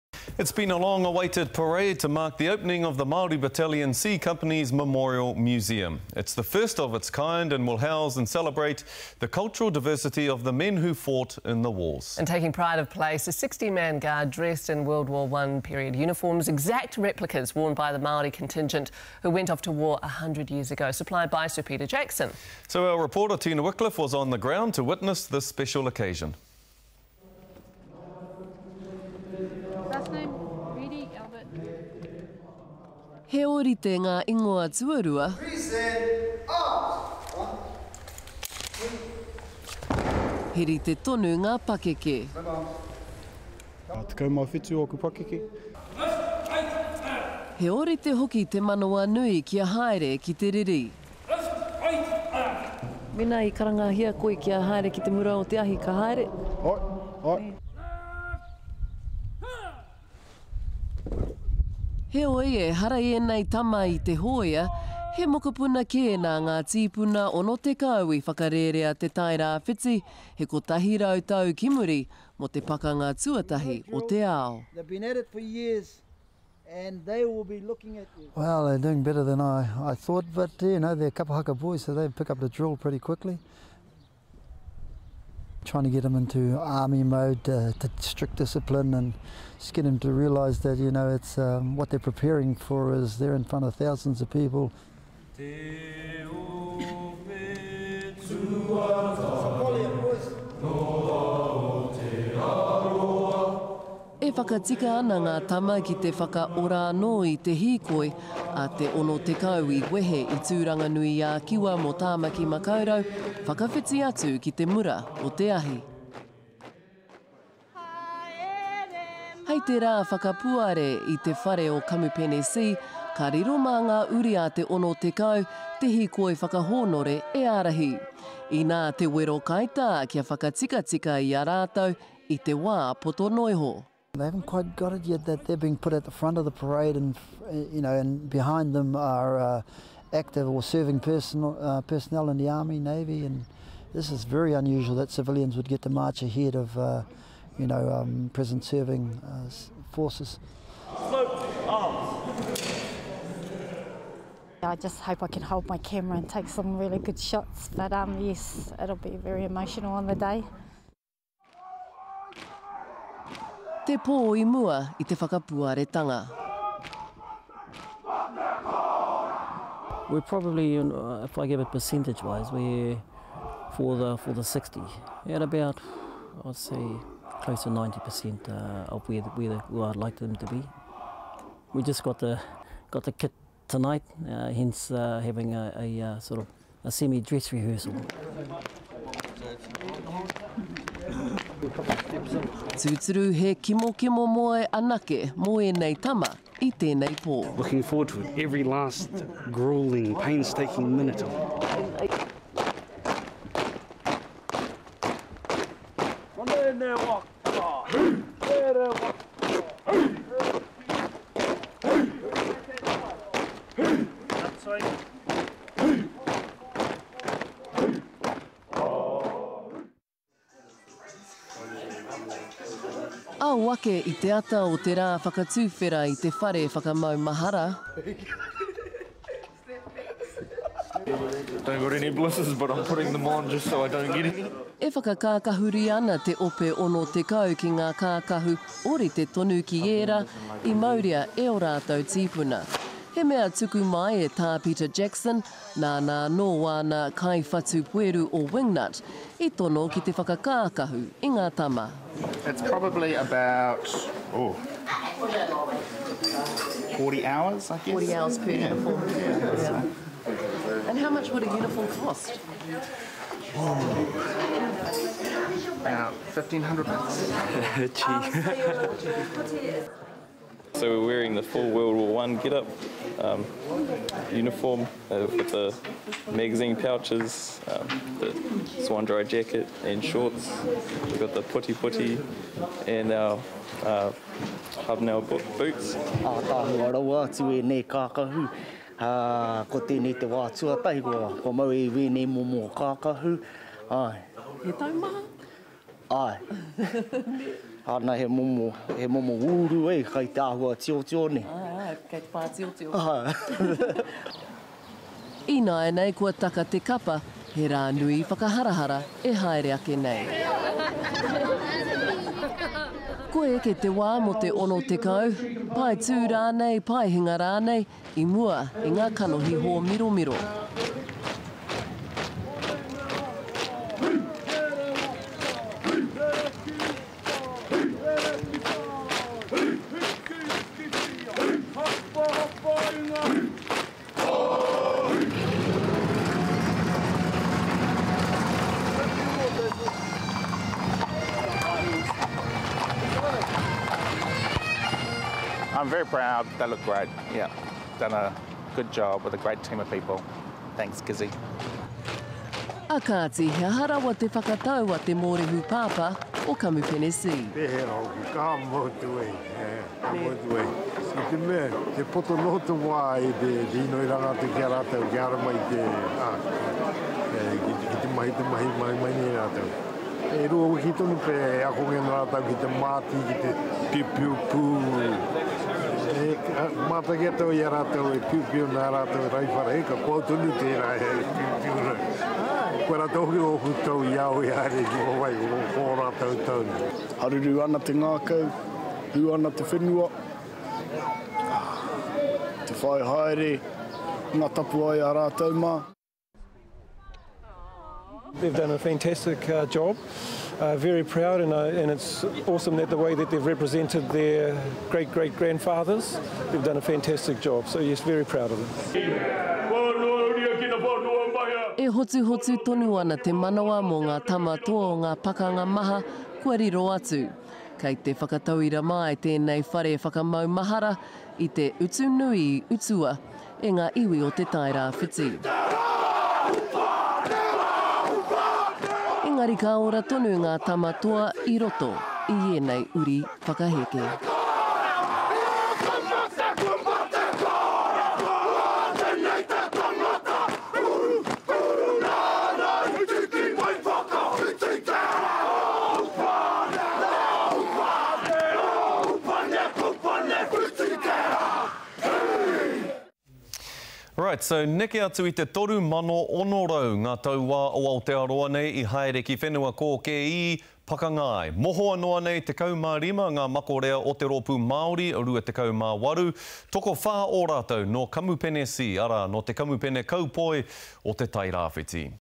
The Price of Citizenship
It's been a long awaited parade to mark the opening of the Māori Battalion C Company's memorial museum.  It’s the first of its kind and will house and celebrate the cultural diversity of the men who fought in the wars. Taking pride of place is a 60-man guard dressed in WW1 period uniforms, exact replicas worn by the Māori contingent who went off to war 100 years ago, supplied by Sir Peter Jackson. Our reporter Tina Wickliffe was on the ground to witness this special occasion.